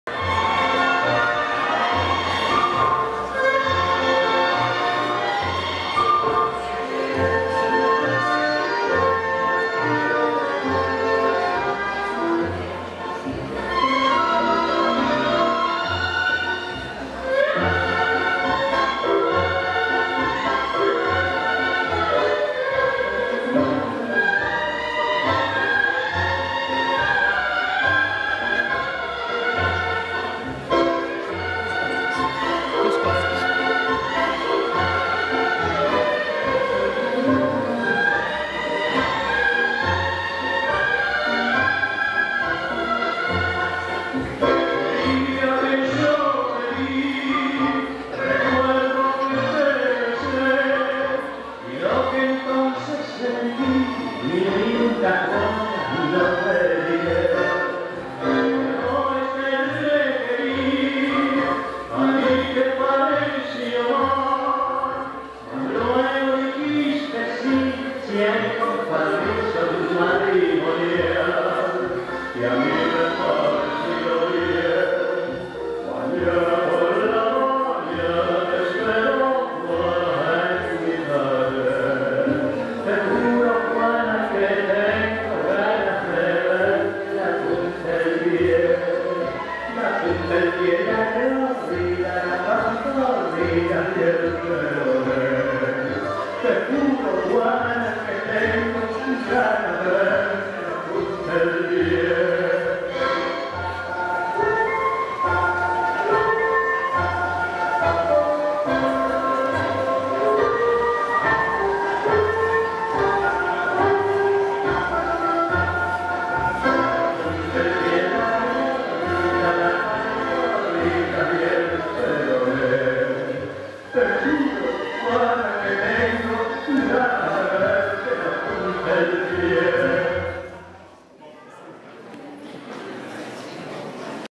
MILONGUEROS ALL ABOARD 2014
Na milonga do adeus.
Na Herança Magana, Domingo 30/11/14, em Vila Nova de Gaia, Portugal.